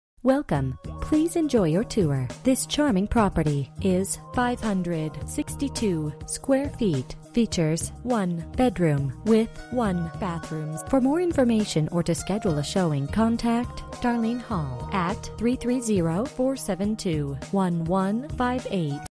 Residential for sale - 473 E Huston Street, Barberton, OH 44203
Rental Property sold alone or with 477 E. Huston Street as a package.  Current tenants pay $380 a month plus utilities

Bedrooms: 1
Bathrooms: 1
Square Feet: 562
Price: $22,900